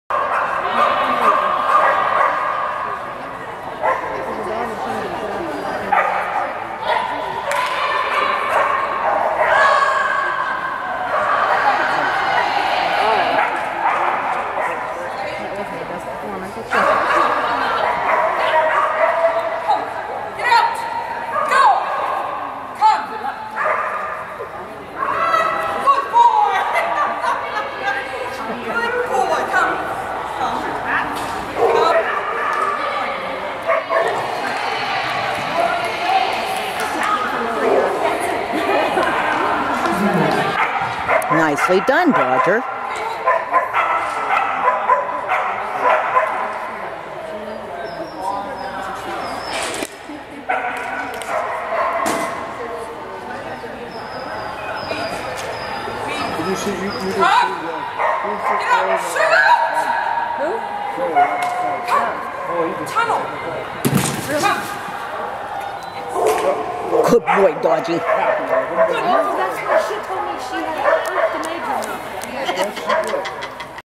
Dodge 7 26 2014
What drama over this run! When I was done I really didn't know if I Q'd. Someone said that I only had a R & W. But the judge came up to the table and changed the R to another W, which is an NQ. The judge is relatively new. Here the leaping over the corner of the aframe confused him. He originally called a refusal (R) but then thought it should be an off course (W) but as you can see he never went off course. So when there was a break, I waived him over and said, "We need to talk." Judge says, "That should be an R, I will change it." I said, "Thank You!" and collected Dodge's second place ribbon and his second leg. WOO HOO!